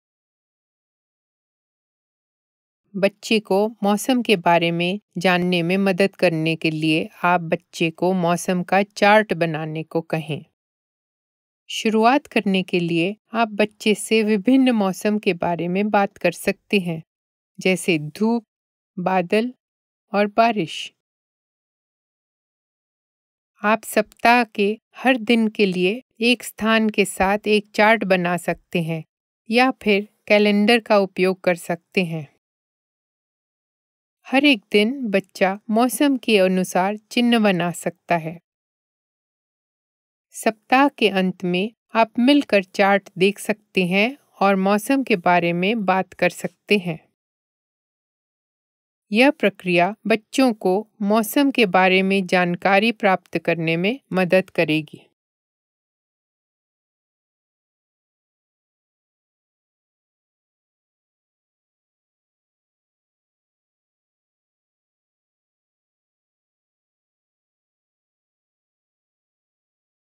मौसम देखना - Watching the Weather (Hindi)
यह प्रक्रिया बच्चो को मौसम के बारे में जानकारी प्राप्त करने में मदद करेगी, यह मौसम में बदलाव के बारे में जानने का एक मजेदार तरीका है।

Hindi translation by Meenakshi Pradhan and narration by Mini Rangrass.

You can help a child learn about weather by making a chart to talk about the different types of weather. This is a fun way to learn about changes in weather.